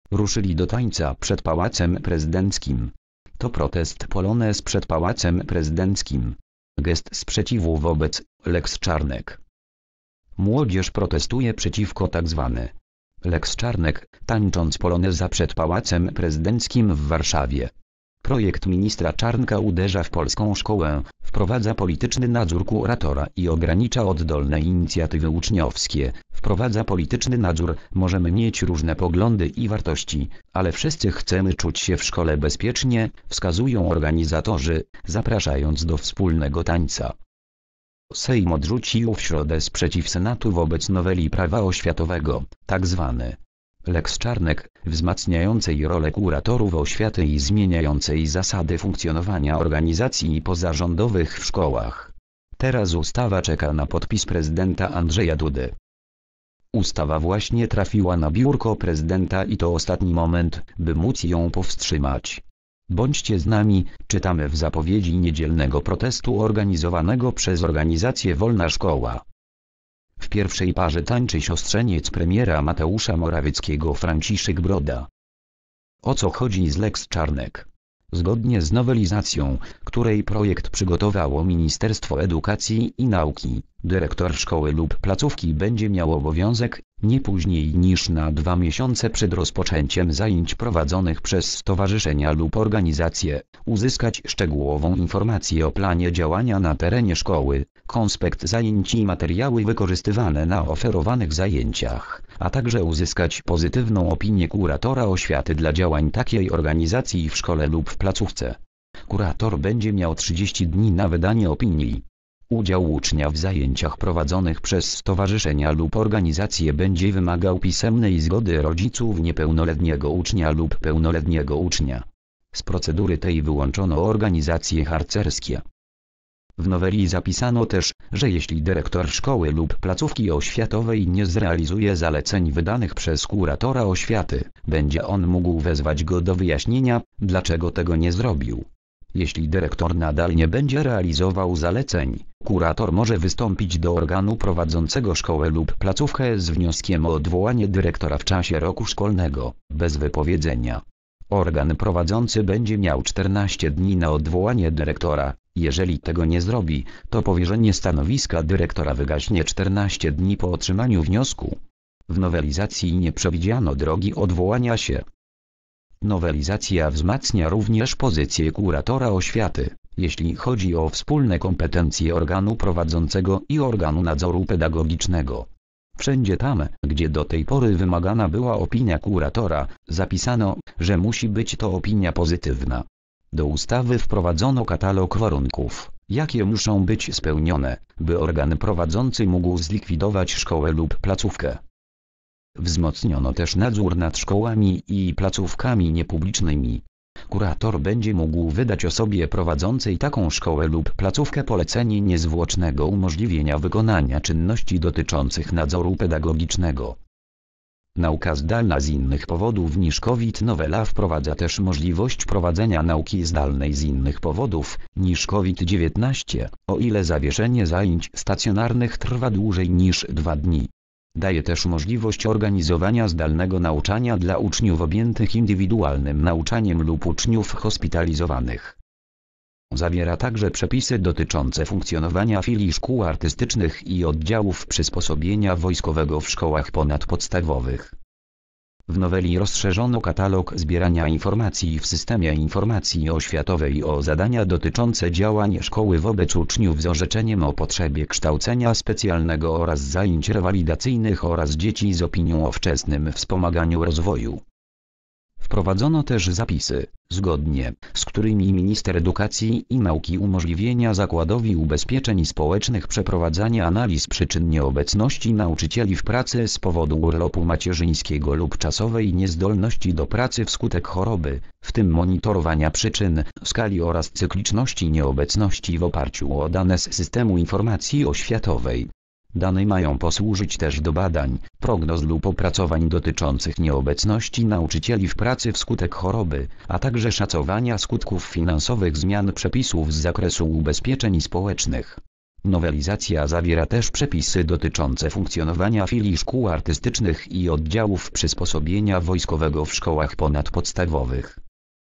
Ruszyli do tańca przed Pałacem Prezydenckim. To protest.
Polonez przed Pałacem Prezydenckim. Gest sprzeciwu wobec "lex Czarnek"
Zobacz nagranie:

   (fot. PAP/Paweł Supernak)